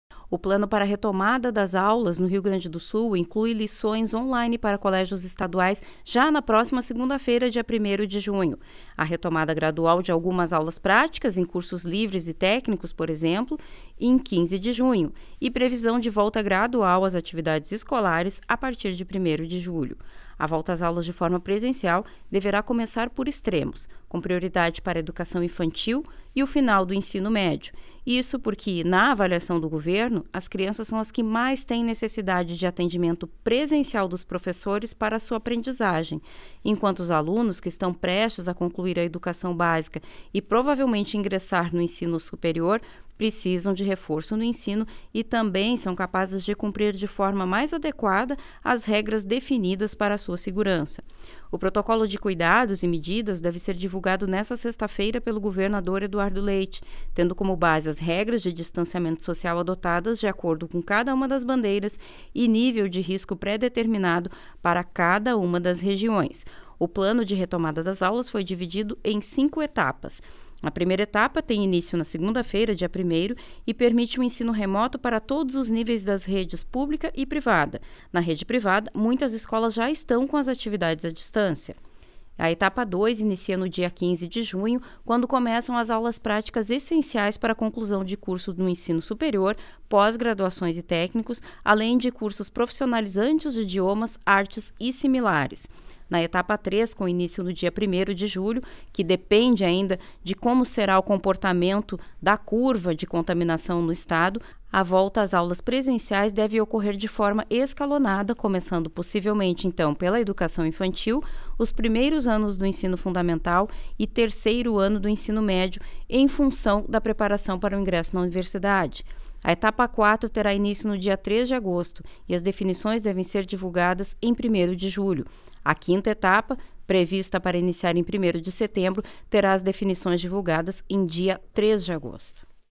Escolas públicas do RS começam aulas remotas na próxima segunda-feira
Aulas serão no formato a distância. Governador anunciou que aulas presenciais não devem retornar antes de julho.